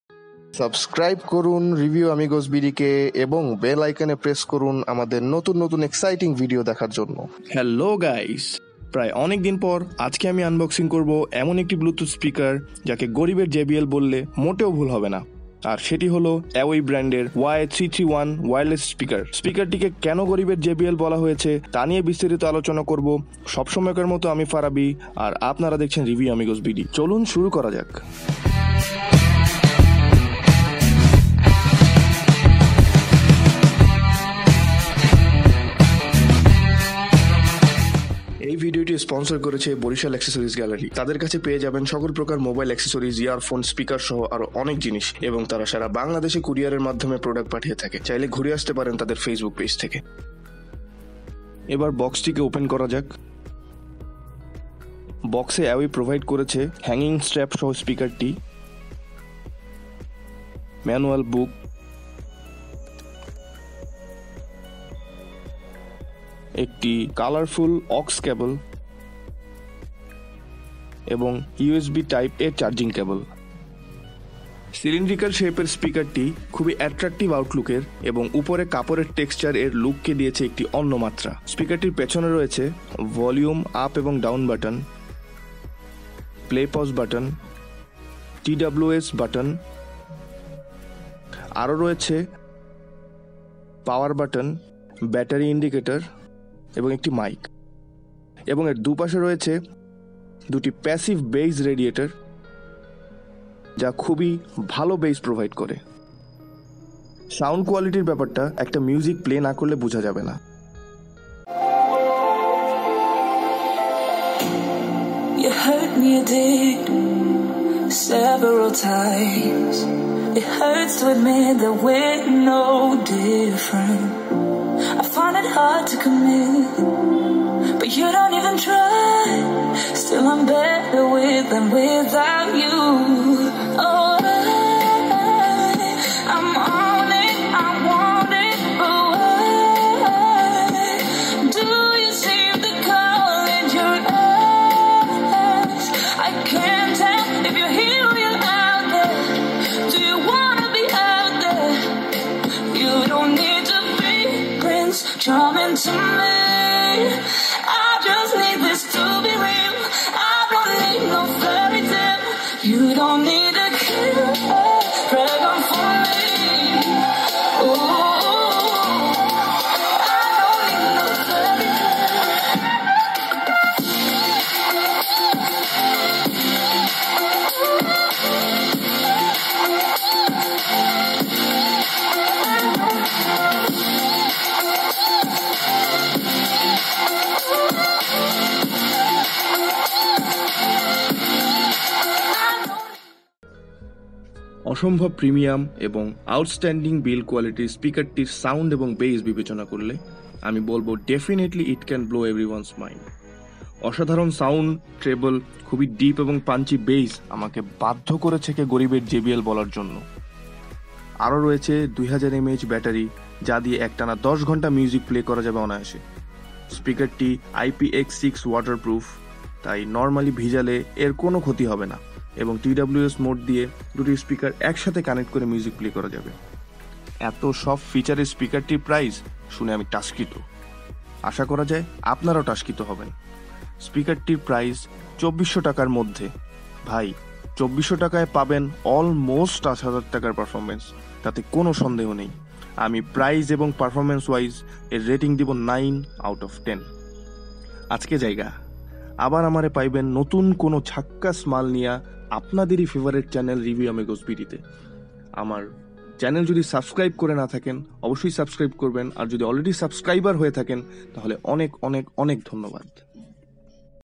AWEI Y331 Bluetooth Speaker Bengali Review || গরীবের JBL || Review Amigos BD
AWEI Y331 Bluetooth Speaker is the most budget friendly speaker we reviewed so far in terms of performance it can compete with brand like JBL but it is much cheaper then any other JBL speaker. AWEI Y331 Bluetooth Speaker is the true budget beast...
Awei Y331 Portable Bluetooth Speaker

    Bluetooth Version:  V4.2+EDR
    Track system:  Stereo（Dual Sound Track ）
    Battery capacity: 2000mAh
    Driver unit: 45mm
    Sensitivity: 150MS
    Impedance: 4Ω
    Rated power: 5W*2
    Driver power: RMS  5W*2
    Charging time: 4h
    Talk time: 10h
    Music playtime: 8-10h
    Bluetooth Protocol: A2DP / AVRCP / HFP / HSP
    Frequency range: 30Hz-20KHz
    Input voltage: DC5V-500mA
    Bluetooth frequency: 2.4GHz-2.480GHz
    Optimal talking distance: 〈1M
    Transmission Distance:  10M(barrier-free space)
    voice prompt: Yes
    multipoint connection: Yes